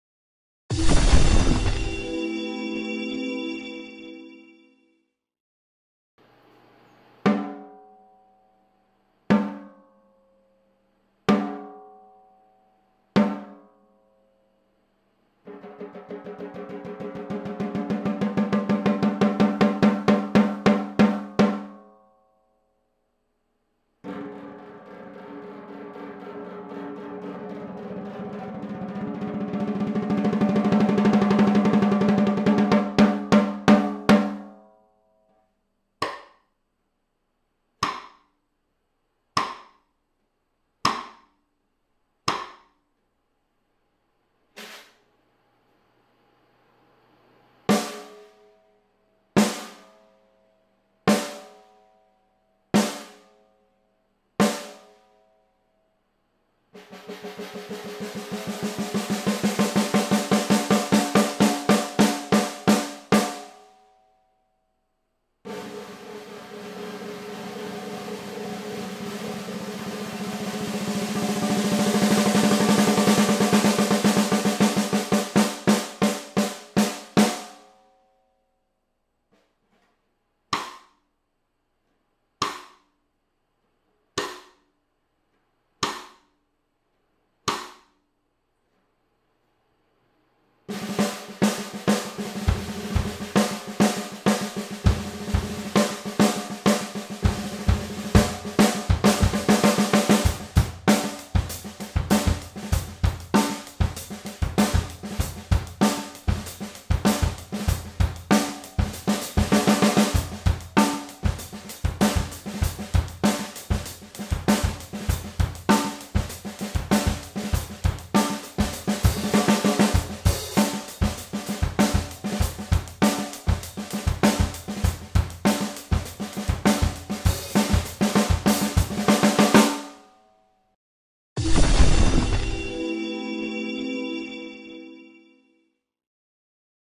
Tama 박철우 Signature 14X5
안녕하세요
드러머 zune 입니다
오늘 소개해드릴 영상은
Tama  박철우 Signature 14X5 모델입니다~